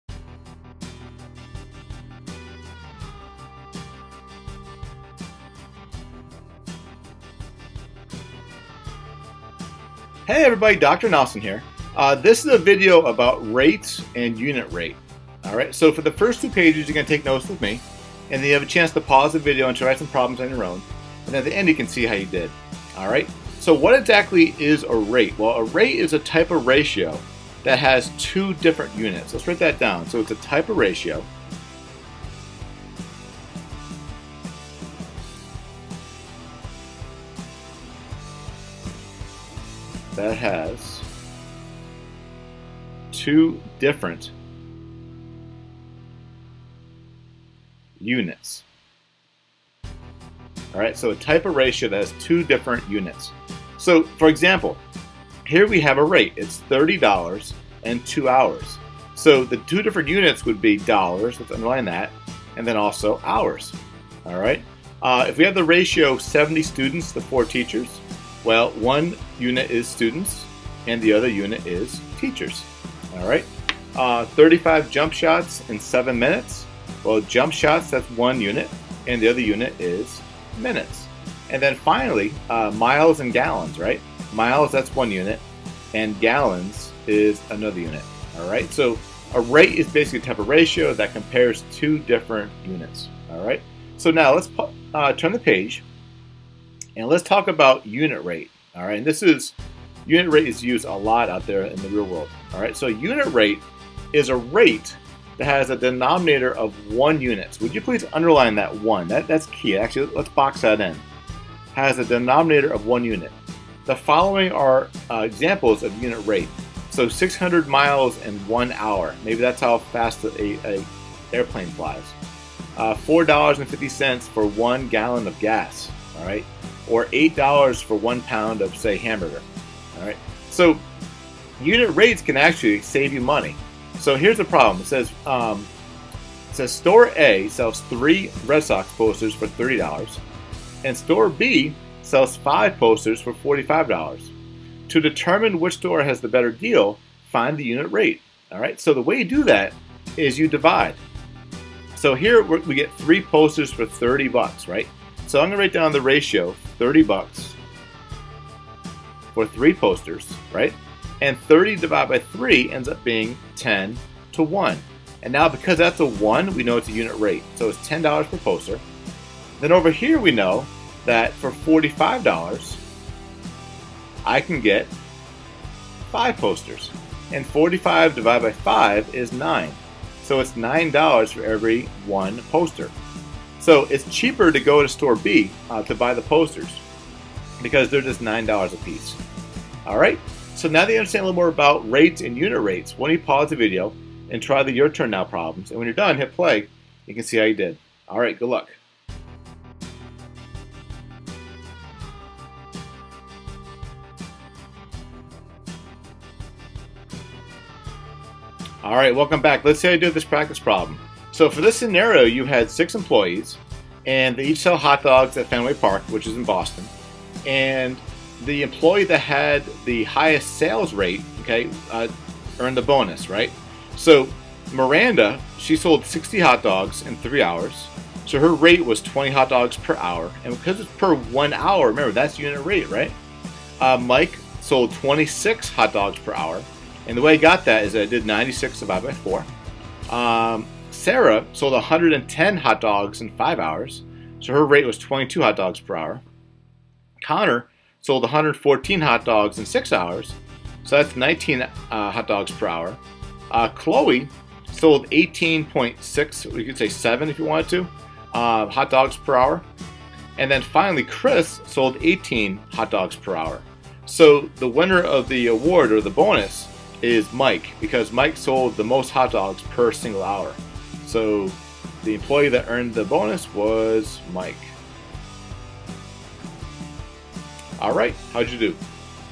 Mastering Rates and Unit Rate with Examples | Math Made Easy
Rates & Unit Rate

In this video, we’ll explore the concepts of rates and unit rate. You’ll learn how to calculate rates and convert them into unit rates, along with practical examples to make these concepts easy to understand and apply.